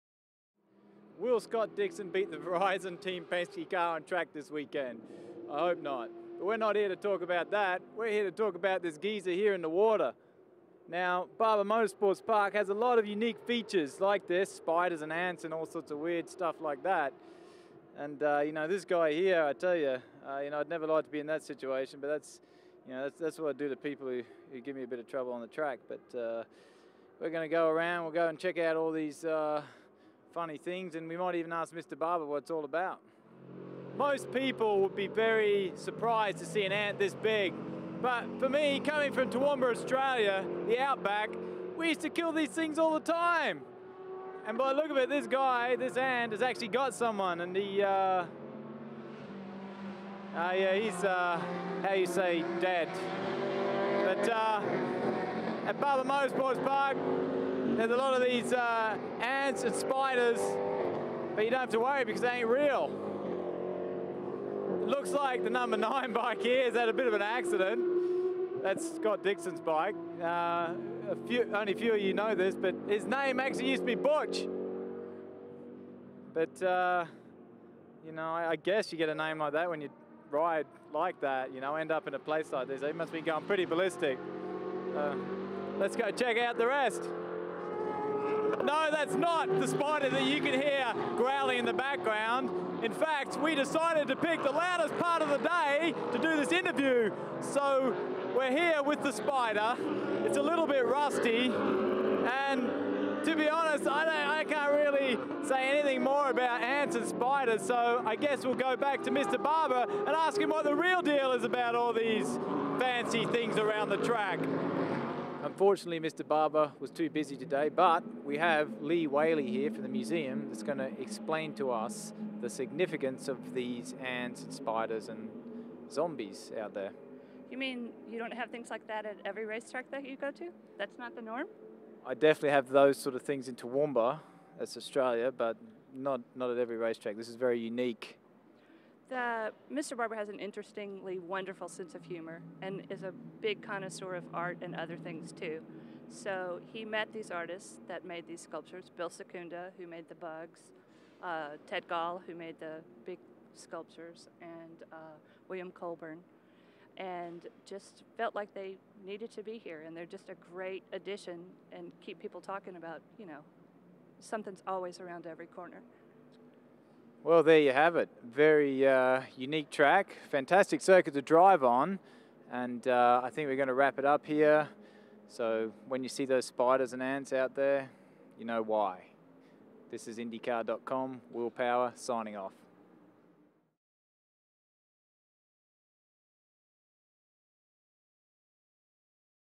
Will Power Barber VBlog
Current IZOD IndyCar Series Points Leader, Will Power, take you around to look at Barber Motorsports Park's unique statues.